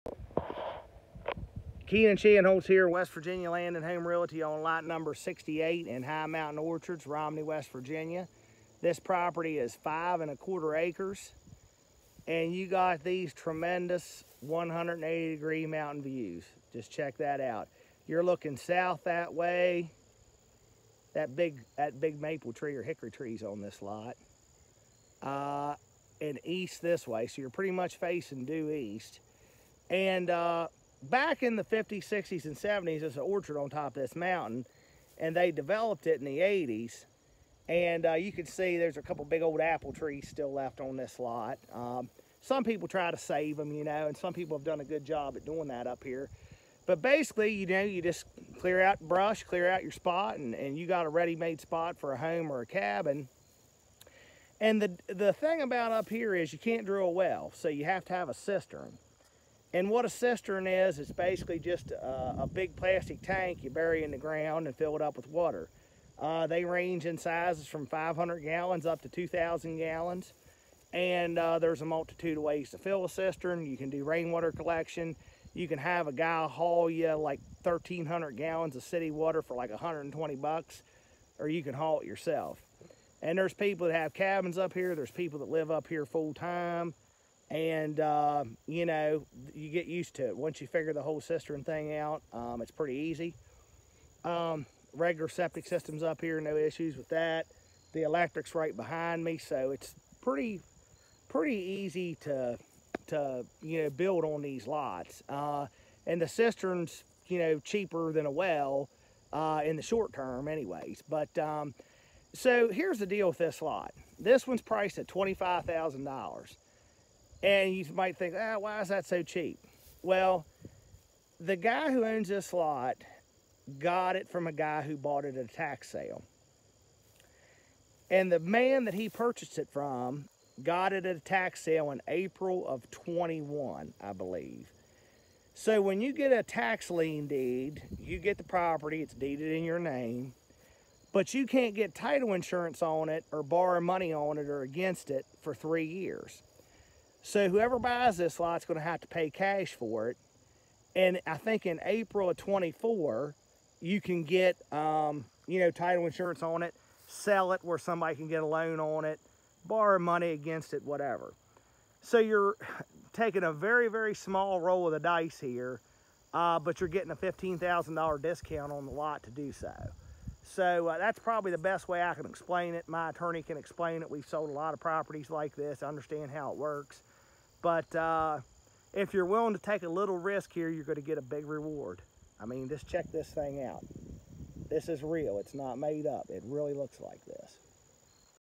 68 High Mtn. Orchards, Romney, WV ~ 5.25 AC ~ $25,000 ***CLOSED 11/30/2022 FOR $25,000***
***CLOSED 11/30/2022 FOR $25,000***
RISK AND REWARD 5 ACRES OF MOUNTAIN BLISS WITH 180 DEGREE PANORAMIC VIEWS TO THE EAST AND SOUTH, OVERLOOKING THE TOWN OF ROMNEY AND SOUTH BRANCH RIVER VALLEY. EASY TO BUILD WITH MINMAL INFRASTRUCTURE COSTS. ONLY 10 MINUTES FROM TOWN, PUBLIC ACCESS ON THE SOUTH BRANCH OF THE POTOMAC RIVER, TROUT FISHING AND PUBLIC HUNTING. $$$$$$$$ CASH ONLY FOR THIS LOT$$$$$$$$$CAN NOT BE FINANCED, WHICH IS WHY IT'S SO CHEAP, CALL FOR DETAILS. THE VIEW IS SO GOOD, YOU JUST GOTTA SEE IT TO BELIEVE IT! ADJACENT LOTS AVAILABLE.